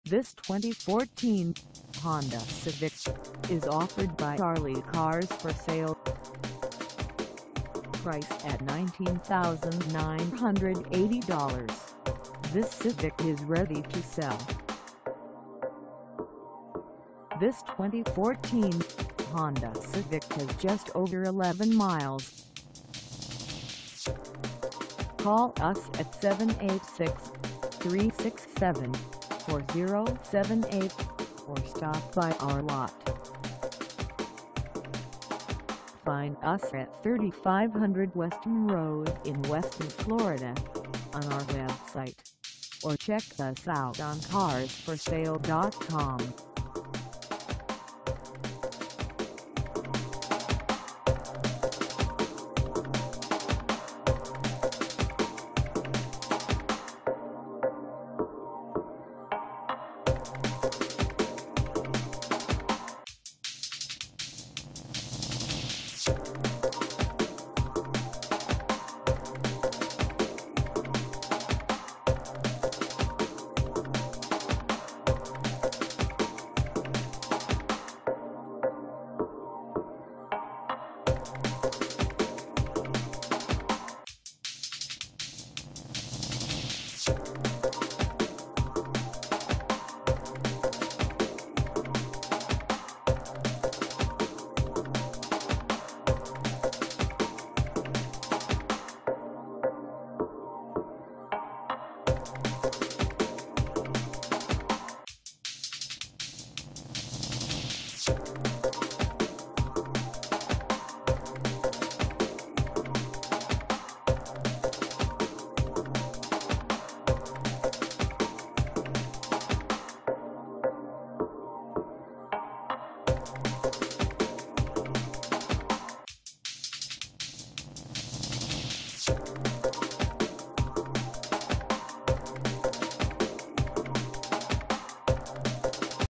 2014 Honda Civic LX 4dr Sedan CVT for sale in WESTON, FL 333
This 2014 Honda Civic LX 4dr Sedan CVT is for sale in WESTON, FL 33331 at Ali Cars For Sale.
Engine: I4 1.8L I4
Transmission: CVT
Color: Unspecified
Mileage: 11
Contact Ali Cars For Sale 3500 Weston Rd WESTON, FL 33331 to test drive this 2014 Honda Civic LX 4dr Sedan CVT today.